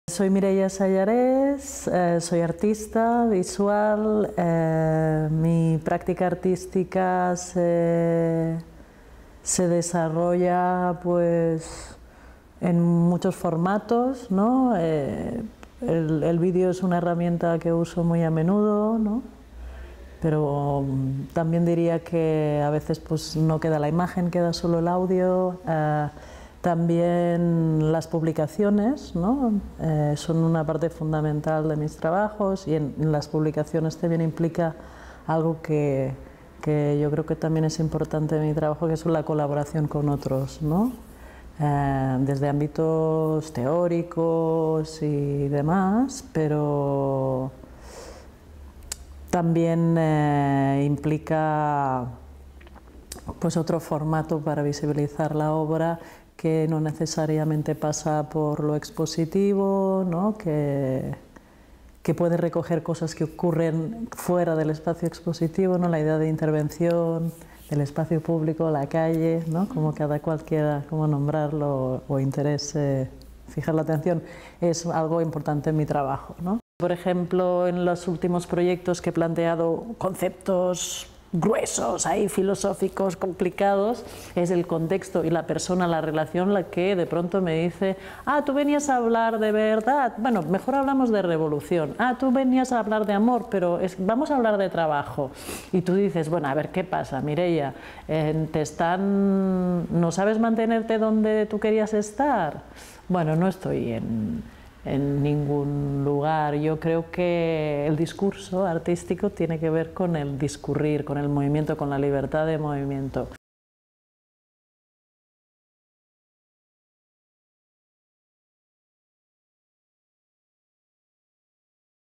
Mireia Sallarés. Programa 09: Íntimo-político
ideo honetan Mireia Sallarésekin izandako elkarrizketaren laburpena ikus daiteke "Programa 09: Intimoa-Politikoa"-ren harira (2015/04/27-29)

En este vídeo se ve el resumen de la entrevista mantenida con Mireia Sallarés en torno a su participación en el Programa 09: Íntimo-Político (27-29/04/2015).
Info +: tabakalera.eu/es/programa/programa09-intimo-politico/